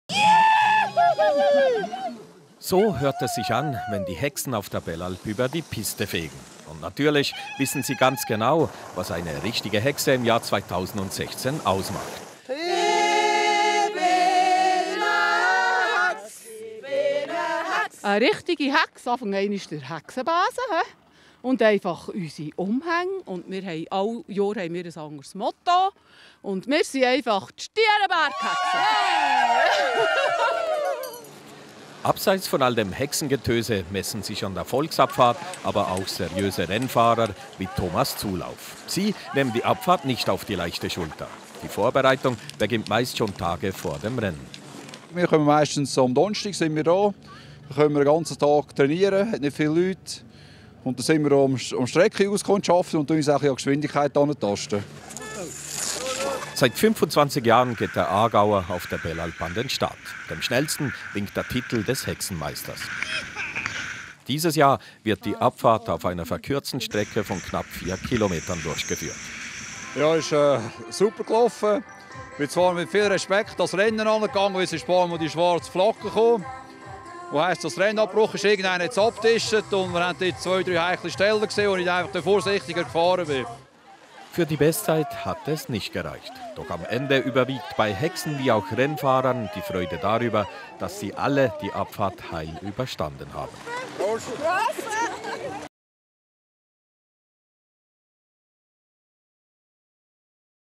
Spellbinding ski race
The witches have been getting up to their old tricks in a traditional downhill ski race at Belalp in Valais. (SRF/swissinfo.ch)